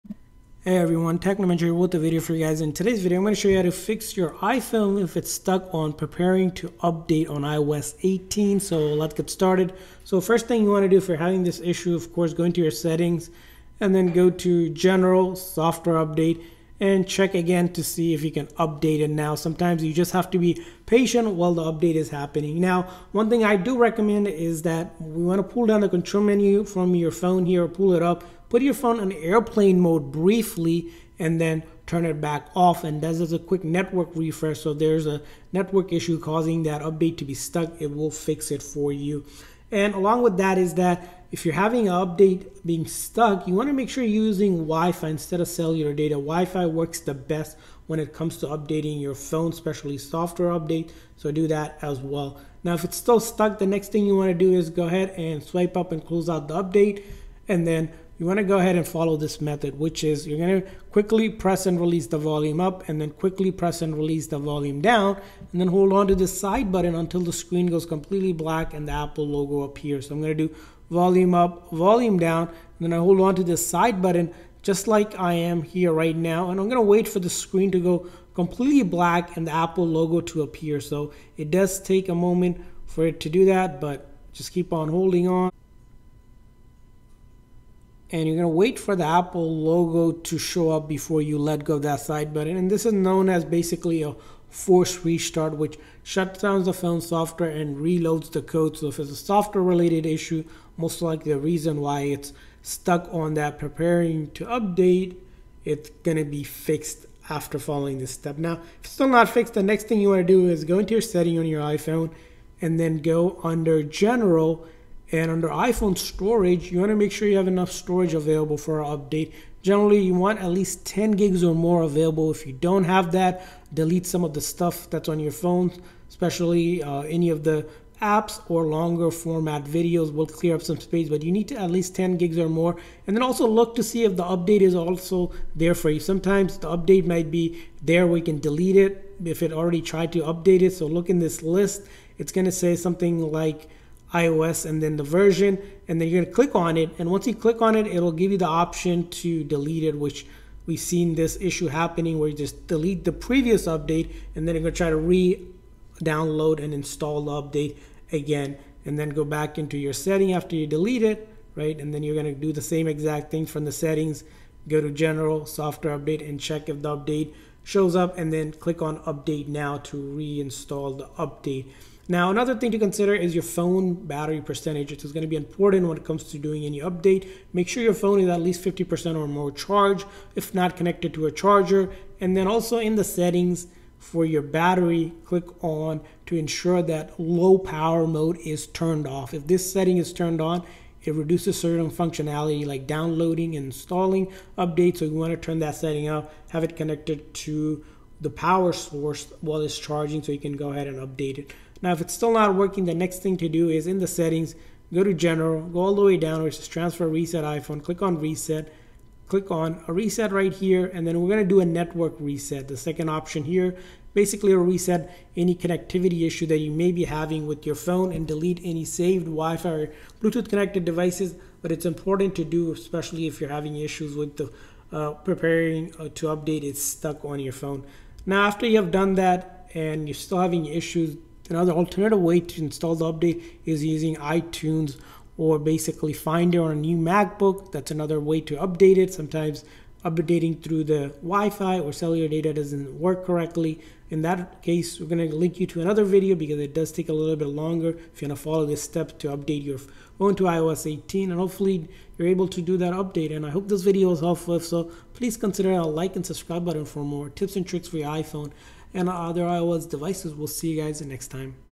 How To Fix iOS 18 Stuck on Preparing Update
Stuck on Preparing Update screen on iOS 18 update? In this video I show you how to fix iPhone stuck on iOS 18 Preparing Update by following these steps!